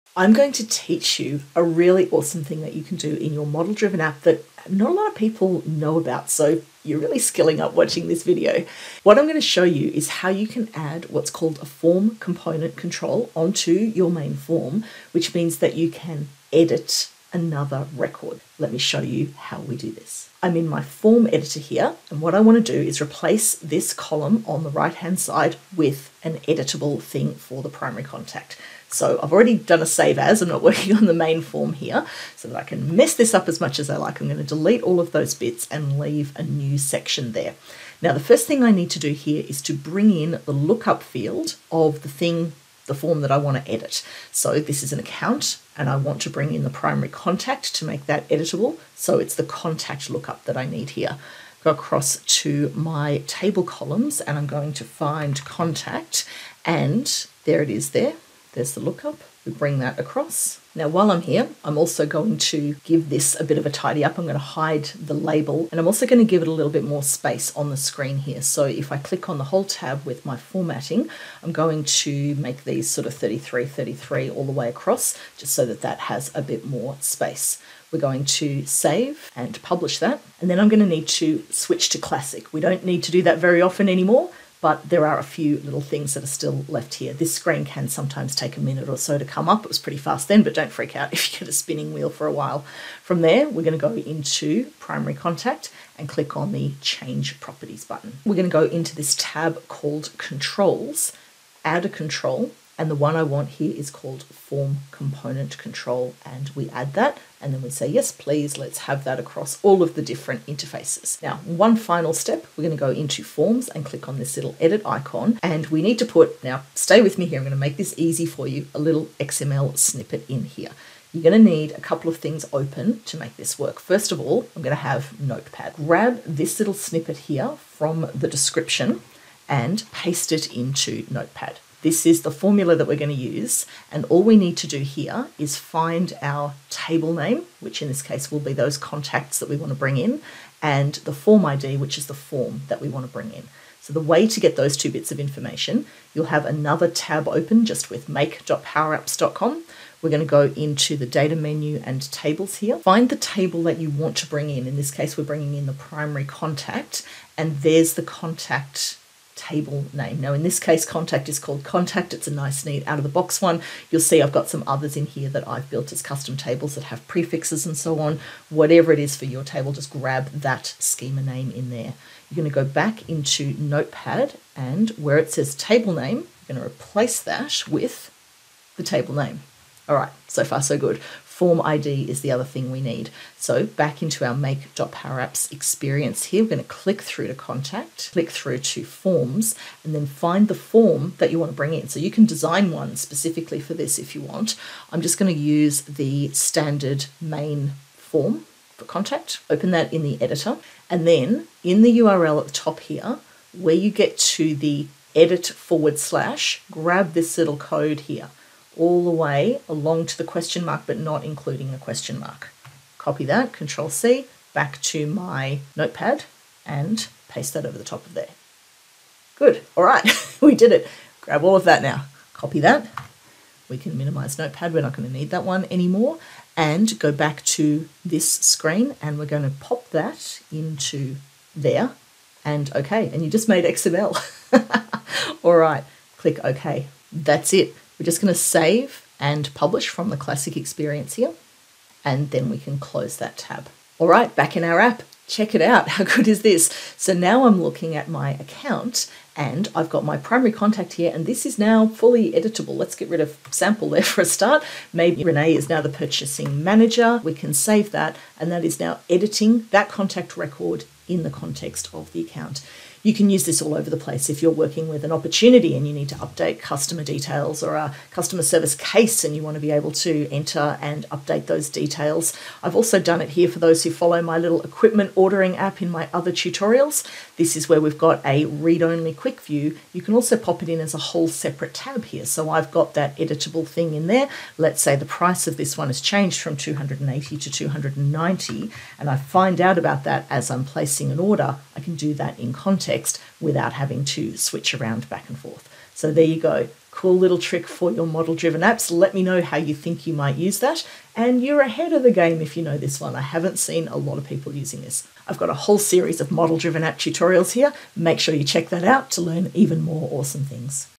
Power Apps Model-Driven Apps: Form Component Control Tutorial
Use a Form Component Control in your Power Apps Model Driven app so that you can edit a related record without leaving your form.

You'll find the formula you need for the XML snippet here (Step 11 in this doc)

If you're a complete beginner with Power Apps model-driven apps, make sure you watch my tutorial for complete beginners first